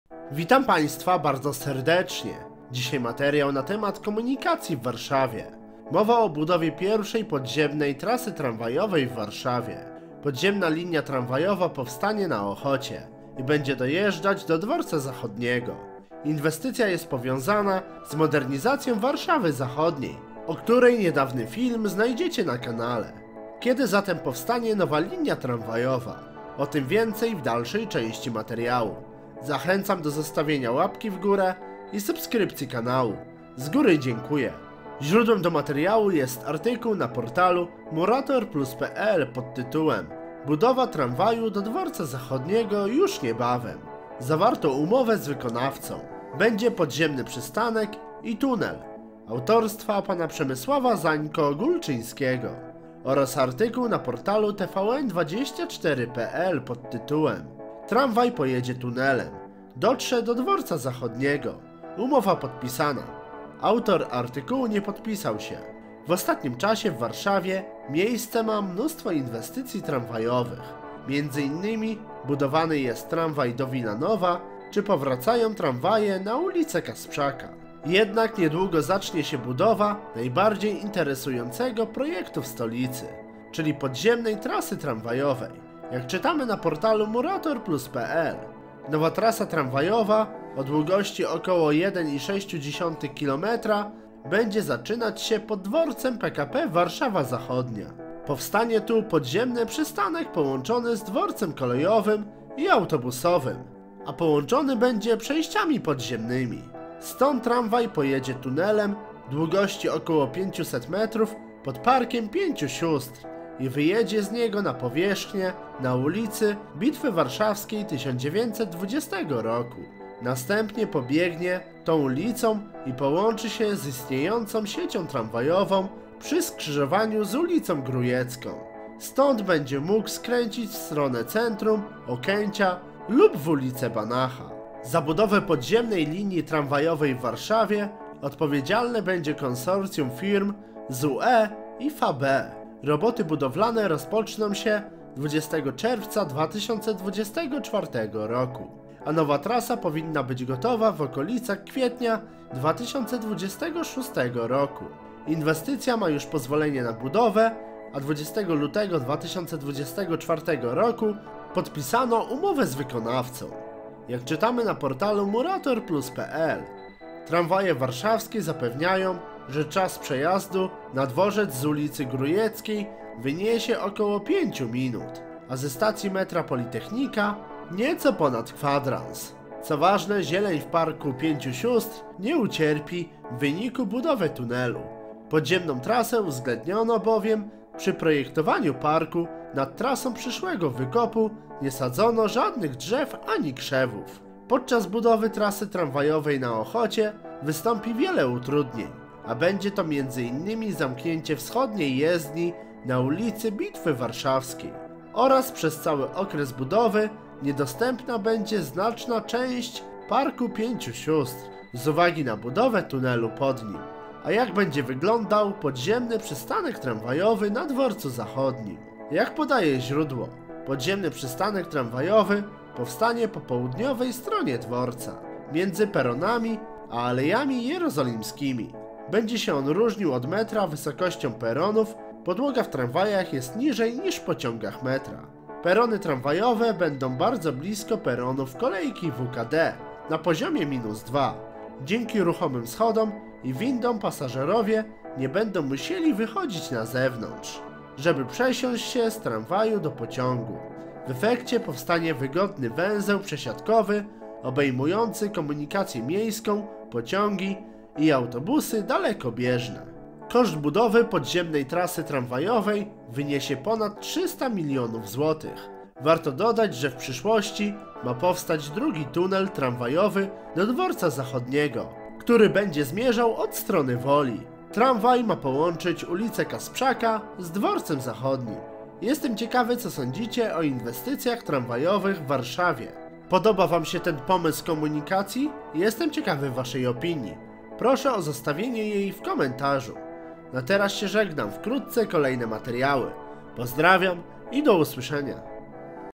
Podziemna Linia Tramwajowa w Warszawie - Podziemny Przystanek na Dworcu Zachodnim! Gdzie Dojedziemy?
Witam Państwa,
Dzisiaj materiał na temat podziemnej linii tramwajowej w Warszawie. Budowa podziemnej linii powiązana jest z modernizacją Warszawy Zachodniej. Tramwaj będzie miał podziemny przystanek na Dworcu Zachodnim. Jaką będzie miał trasę i co z komunikacją miejską w tym miejscu?

Źródła:
Podane na materiale.